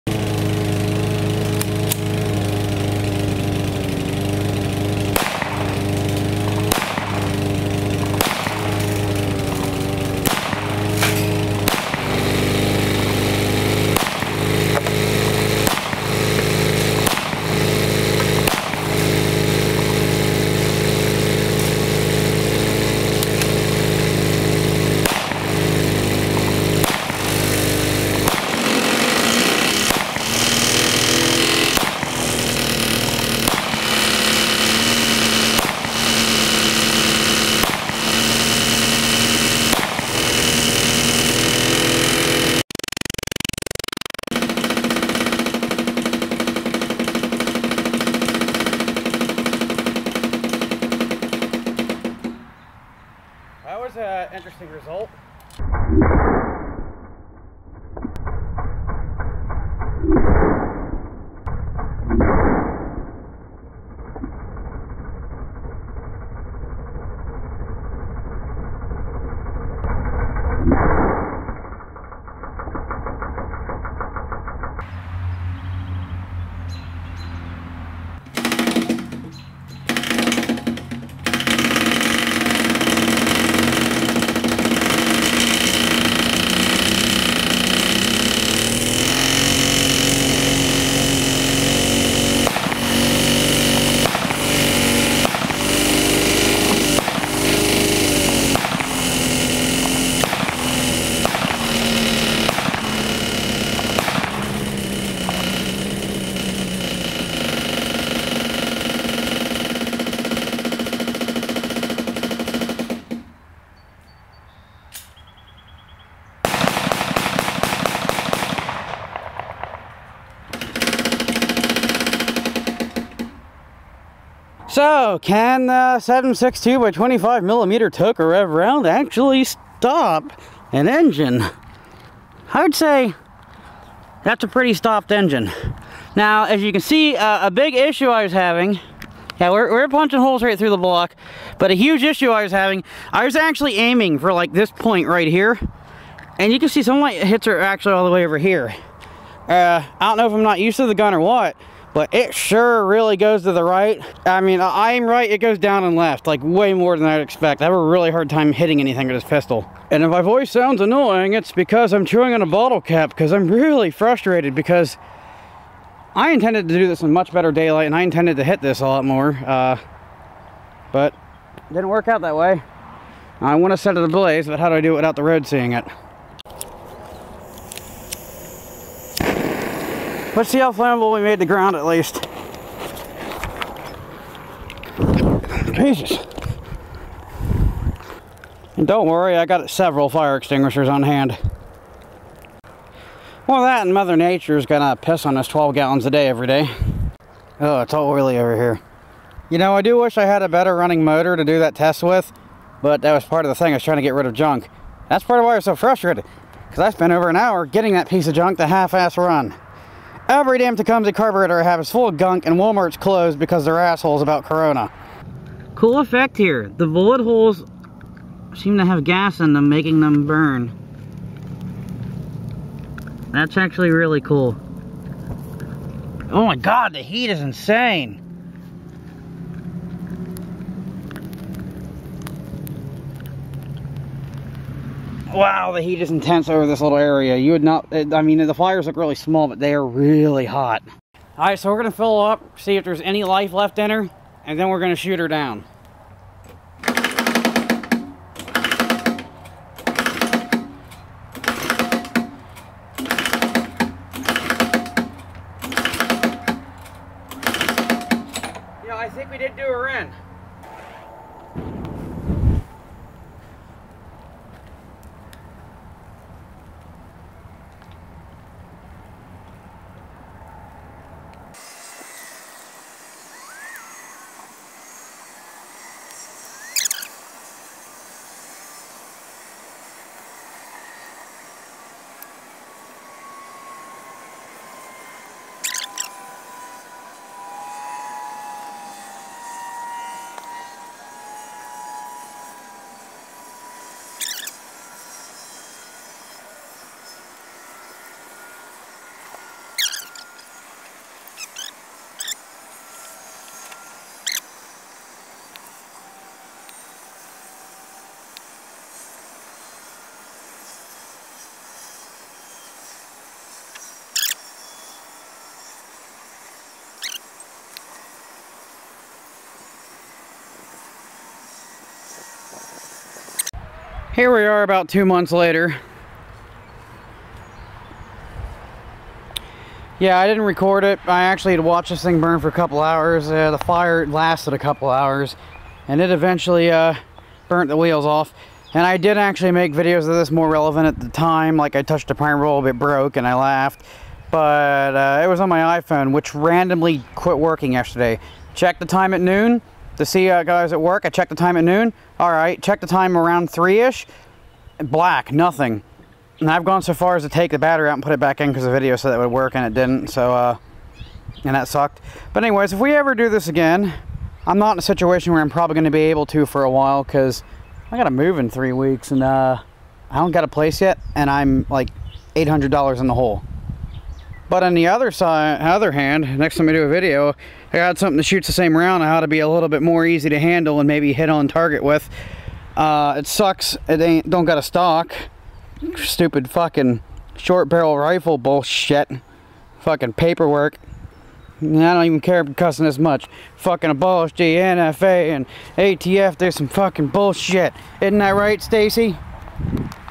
Can 7.62 Tokarev Stop An Engine? (Happy 4th)
hopefully i end up with some land and can keep making videos like this, but shits not looking good. my small eng sales have stopped.